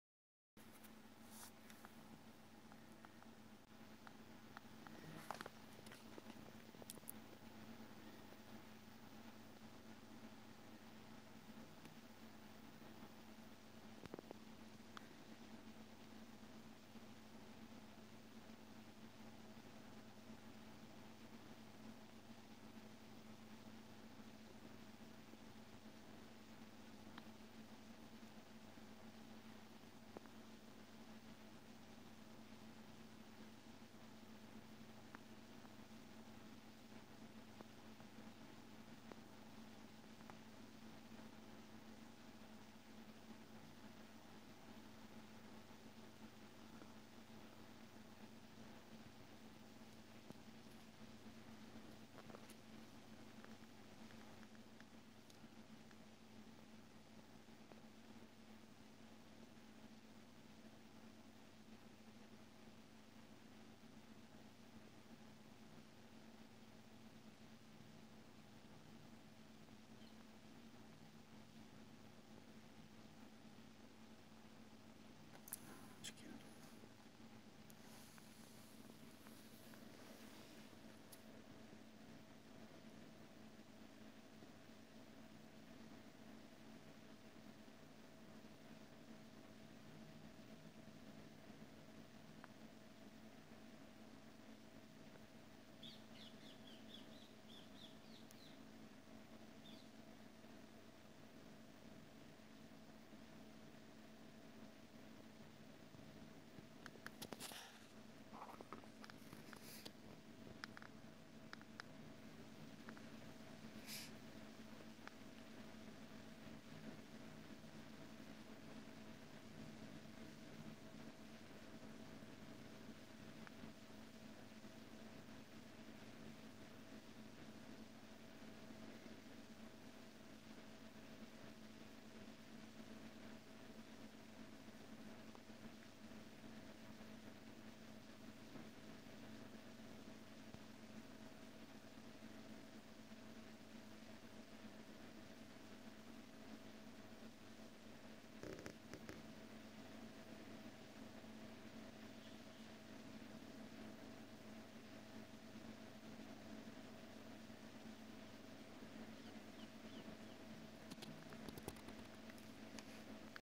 Magpies vs Fox the battle for the chicken
Cheeky pica picas gatecrashing the foxes chicken party